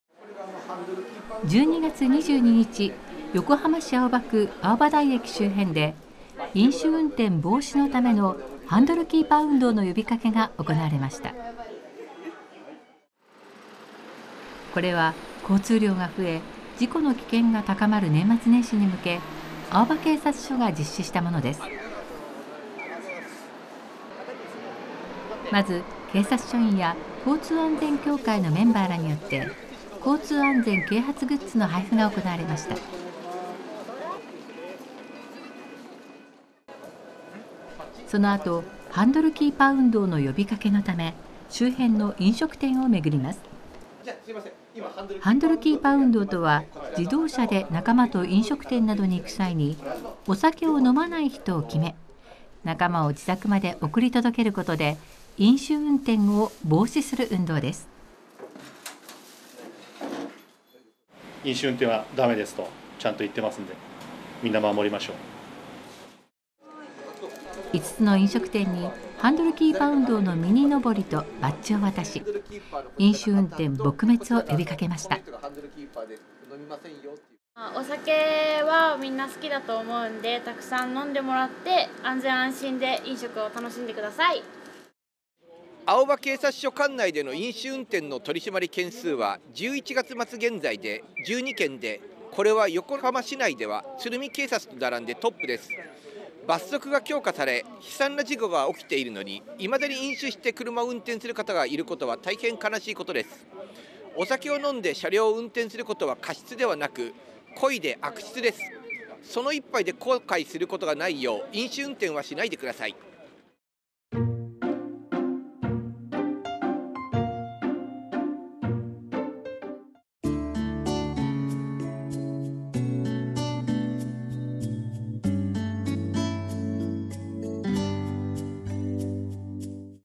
「ハンドルキーパー運動」の呼びかけ【地モトNEWS】2024/1/8放送
「横浜市青葉区」の【「ハンドルキーパー運動」の呼びかけ】をお届けします！
■「ハンドルキーパー運動」の呼びかけ　■場所：横浜市青葉区

12月22日、横浜市青葉区 青葉台駅周辺で飲酒運転防止のための、ハンドルキーパー運動の呼びかけが行われました。

これは、交通量が増え、事故の危険が高まる年末年始にむけ青葉警察署が実施したものです。まず、警察署員や交通安全協会のメンバーらによって、交通安全啓発グッズの配布が行われました。そのあと、「ハンドルキーパー運動」の呼びかけのため、周辺の飲食店をめぐります。「ハンドルキーパー運動」とは、自動車で仲間と飲食店などに行く際にお酒を飲まない人を決め、仲間を自宅まで送り届けることで飲酒運転を防止する運動です。

「飲酒運転はダメですと、（店としても）ちゃんと言ってますんで。みんな守りましょう。」

5つの飲食店にハンドルキーパー運動のミニのぼりとバッジを渡し、飲酒運転撲滅を呼びかけました。

「お酒はみんな好きだと思うんで、たくさん飲んでもらって、安全安心で飲食を楽しんでください。」

■青葉警察署 交通課長 杉山裕紀さん
「青葉警察署管内での飲酒運転の取り締まり件数は11月末現在で12件で、これは横浜市内では鶴見警察と並んでトップです。罰則が強化され悲惨な事故が起きているのに未だに飲酒して車を運転する方がいることは大変悲しいことです。お酒を飲んで車両を運転することは過失ではなく、故意で悪質です。その一杯で後悔することがないよう、飲酒運転はしないでください。」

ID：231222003